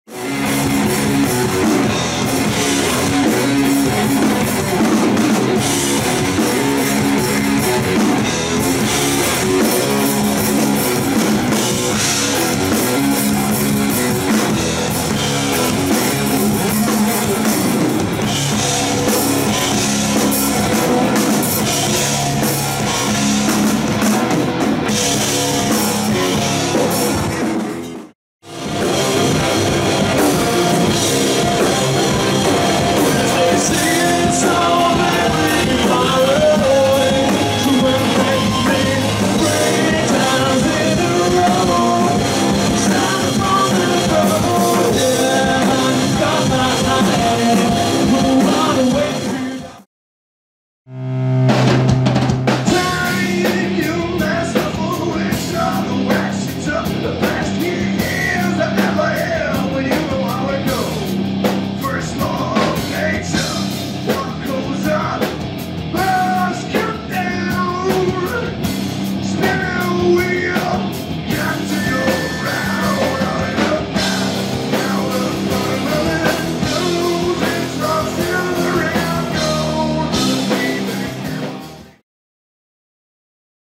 Plainride, The Trikes, Fire Down Below
Live @ WILLEM TWEE  POPPODIUM Den Bosch, The Netherlands
Filmed by the WILLEM TWEE video crew